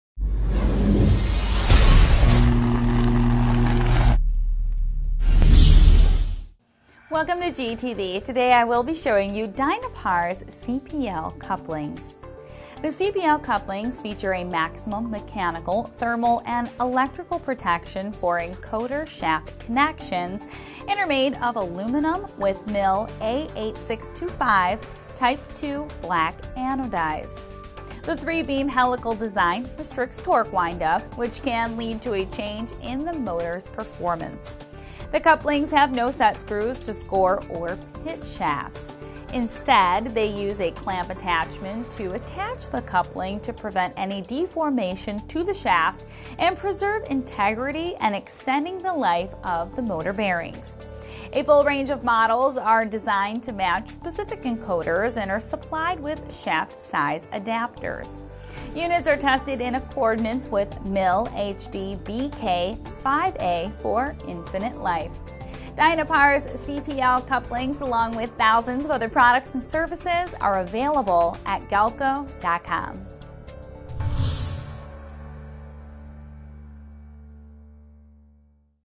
Dynapar CPL Couplings
Dynapar CPL Couplings presented by Katie Nyberg for Galco TV.

Dynpar CPL series designed with maximum mechanical, thermal, and electrical protection for the shaft connections. With the three‑beam helical design it will restricts torque windup. It comes in full range of models designed to match specific encoders are supplied with shaft size adapters.

Features:
-    Three Beam Helical Design
-    No need for setscrews
-    Clamp Attachment